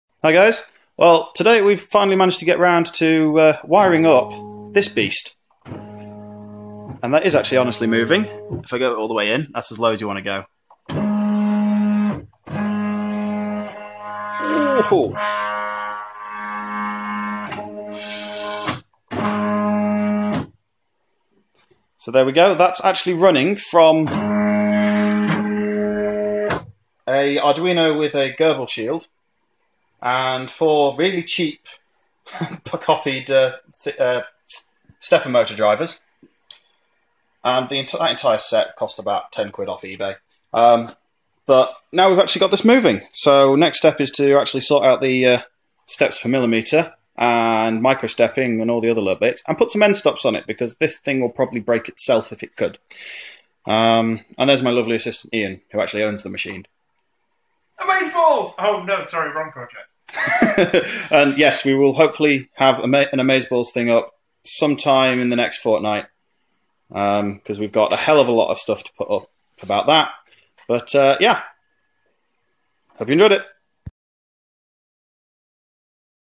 CNC Machine - Quick Update
A quick update of the status of our CNC Machine! We finally have it moving.

This machine is an Ooznest OX CNC Machine, with a bed size of 1m x 750cm.